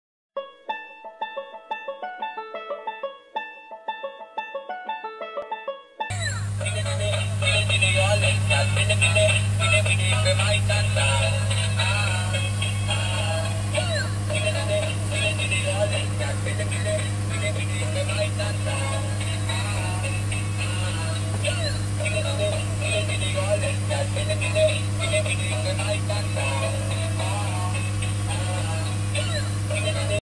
Clownfish Bump & Go With Lights & Sound Battery Operated Toy (ZR143-1)
Clownfish toy is enabled with Lights, Musical Sound, 360 Degree rotation.
Bump and Go Action, runs on smooth surfaces - It will change directions and run when it bumps into something 360 degree rotation.
The attractive make and function of this toy car can engage your child playing and having fun in leisure. Fun car Toy with Lights and sounds Children's Kids Toy - Battery Operated.